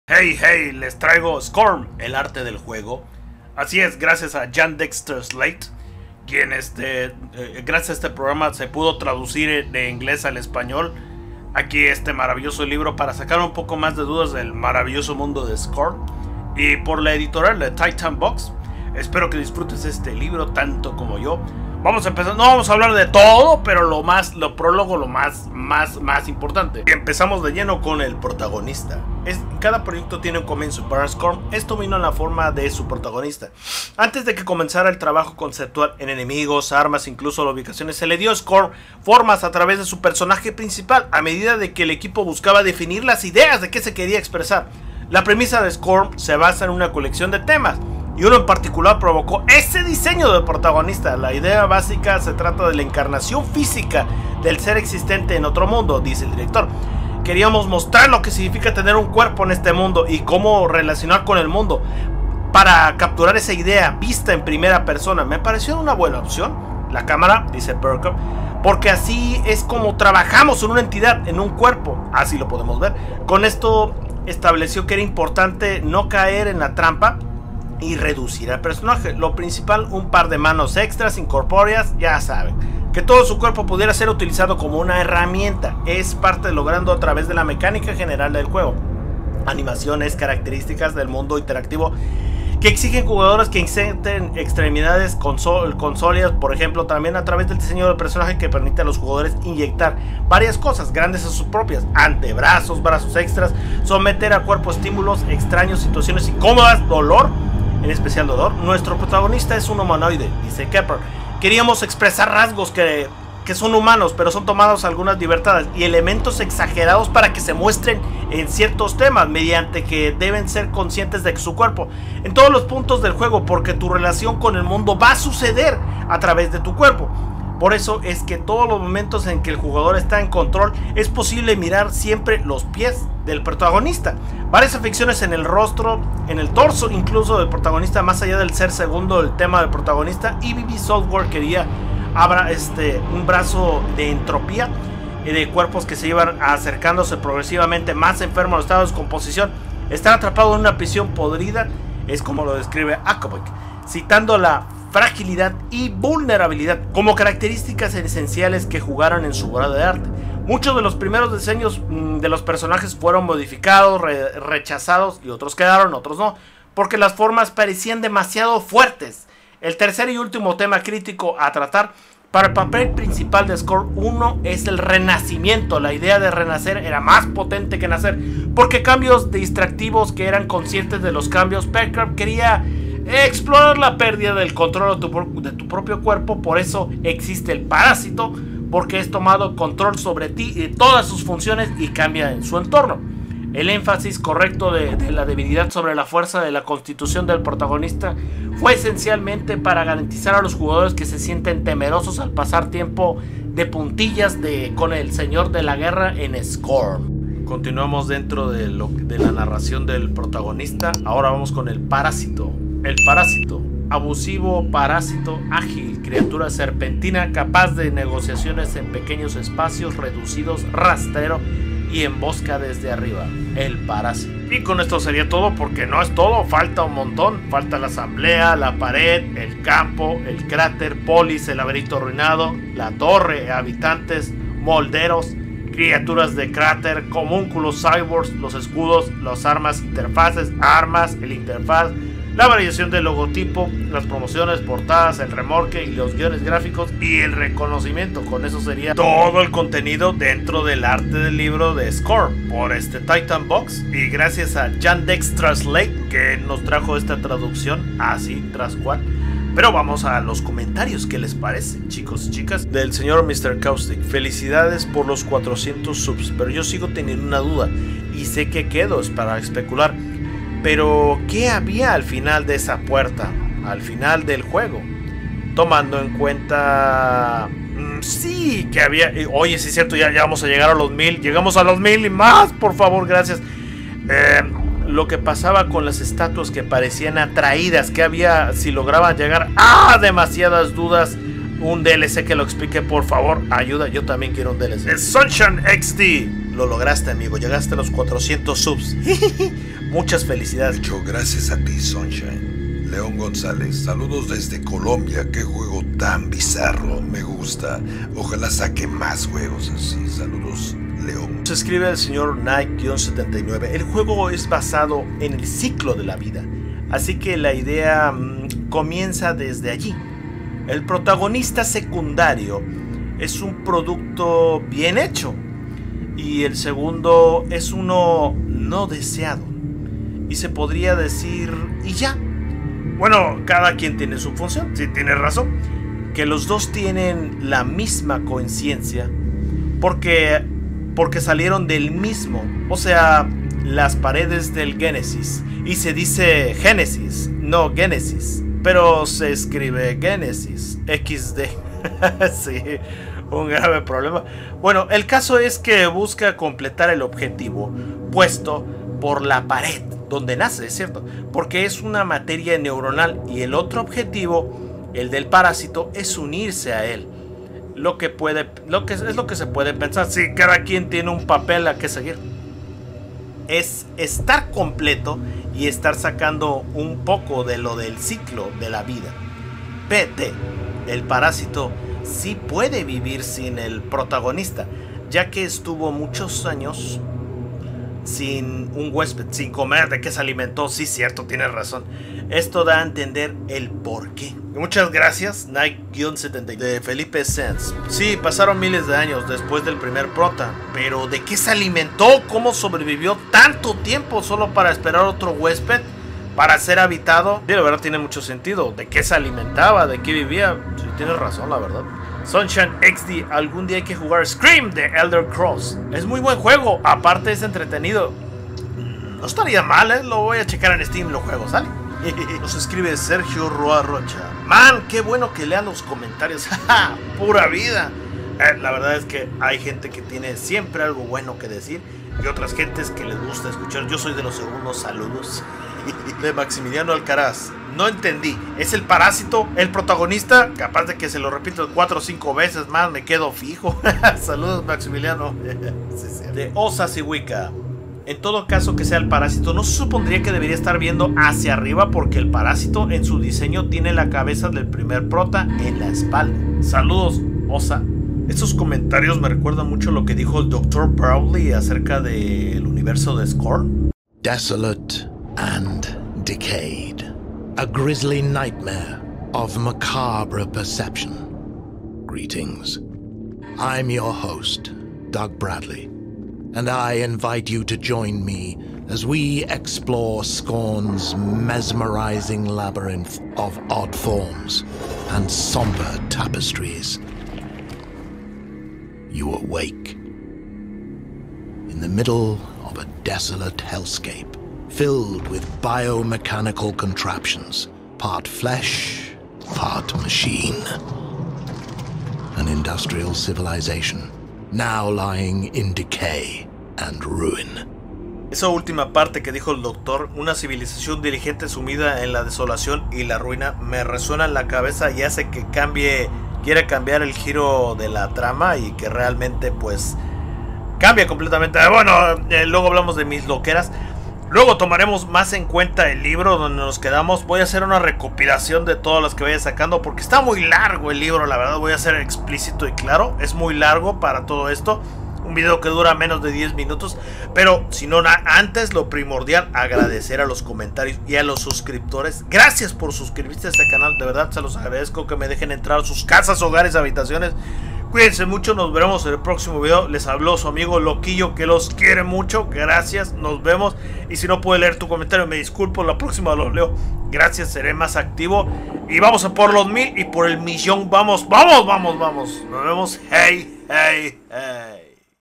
scorn el arte del juego parte 1
En esta ocasión leeremos el libro de; scorn el arte del juego, gracias a la Aplicación; Yandex tralate,  podremos saber mas de este maravilloso mundo de scorn y sus cesos y demás, hey hey hey vamos por los 1000 gracias mi Gente por su suscripción